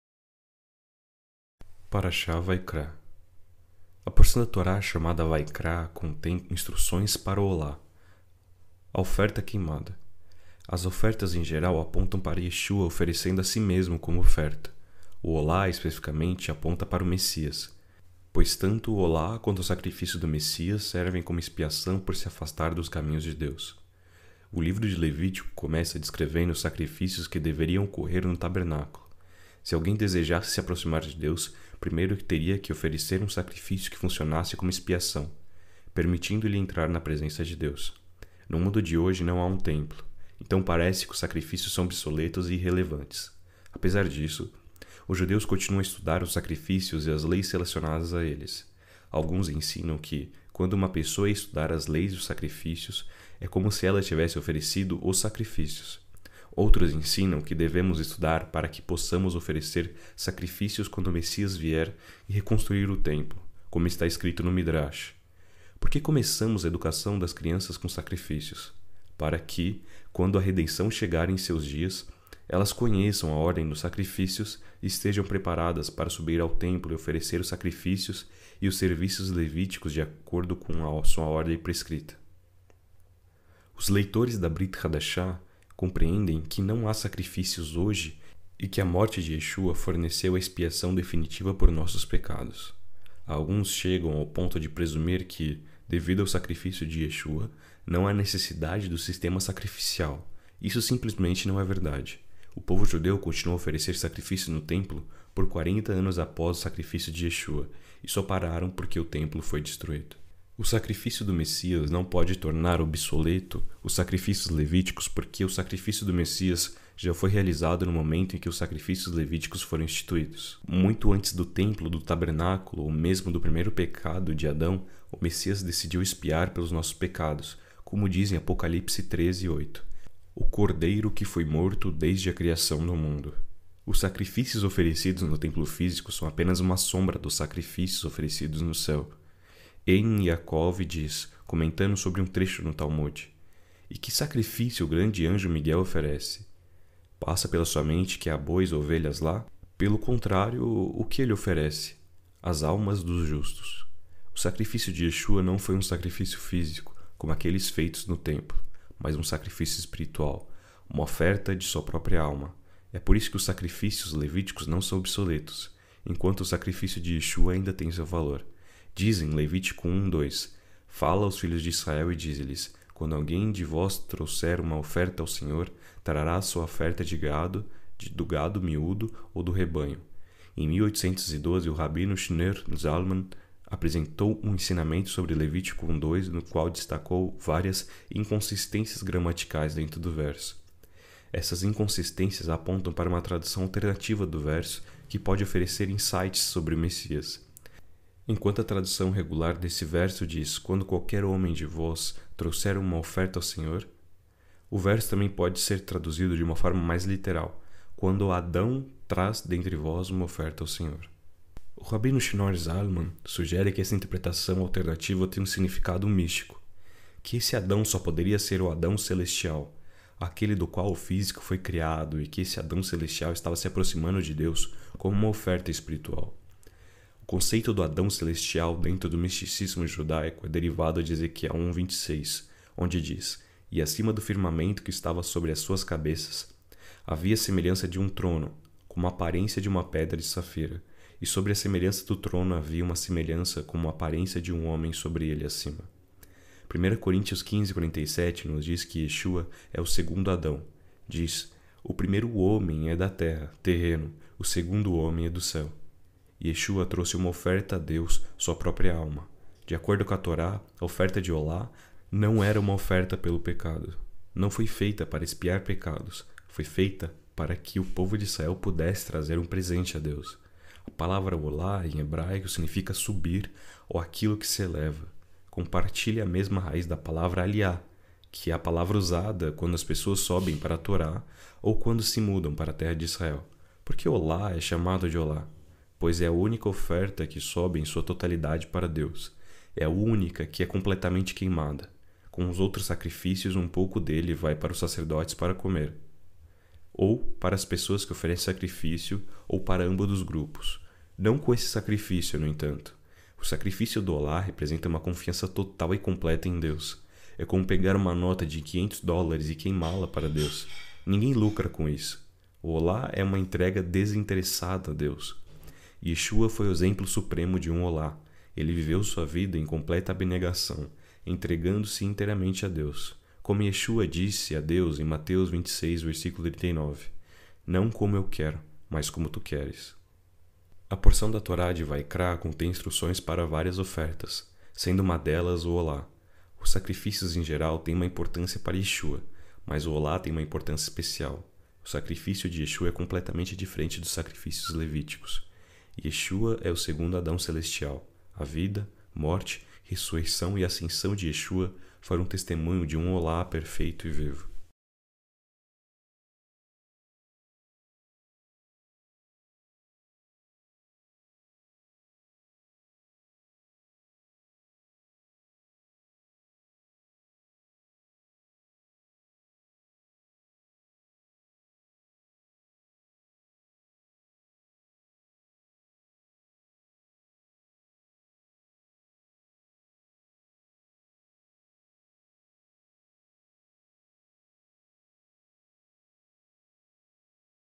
Yeshua na Torá - PARASHÁ VAYIKRA - Messianic Jewish Bible Study
Vayikra: O Significado Profundo do Sacríficio de Yeshua e o Olah na Torá

A porção da Torá de Vayikra revela uma riqueza de significados por trás dos sacrifícios, especialmente o Olah, e sua conexão com Yeshua como o Messias. Junte-se a nós nesta exploração das Escrituras Sagradas e descubra como o sacrifício de Yeshua ecoa os princípios do Olah, oferecendo uma compreensão mais profunda do plano divino para a humanidade.

Entenda como os sacrifícios descritos em Levítico apontam para Yeshua como a oferta definitiva, proporcionando expiação pelos pecados e abrindo o caminho para a reconciliação com Deus. Explore as nuances do Olah e sua simbologia, que transcende o aspecto físico do sacrifício para representar uma entrega total e completa a Deus.

Embora alguns possam considerar os sacrifícios obsoletos, mesmo após o sacrifício de Yeshua, é crucial compreender a continuidade e relevância dos princípios por trás deles. Descubra como os sacrifícios levíticos e o sacrifício de Yeshua são interligados, cada um desempenhando um papel único no plano redentor de Deus para a humanidade.

Explore as interpretações místicas e proféticas relacionadas ao Olah, incluindo a sugestão de que o sacrifício de Yeshua reflete o conceito de um Olah perfeito e vivo, exemplificado por sua total entrega a Deus. Delve into the rabino Shneur Zalman's insights on Leviticus 1:2 and its connection to the celestial Adam, shedding light on Yeshua's role as the second Adam.

Venha conosco nesta jornada de descoberta espiritual enquanto desvendamos os mistérios do sacrifício de Yeshua e sua profunda ligação com os princípios do Olah na Torá. Prepare-se para uma compreensão mais profunda do plano divino para a redenção da humanidade e o papel central de Yeshua como o Messias prometido.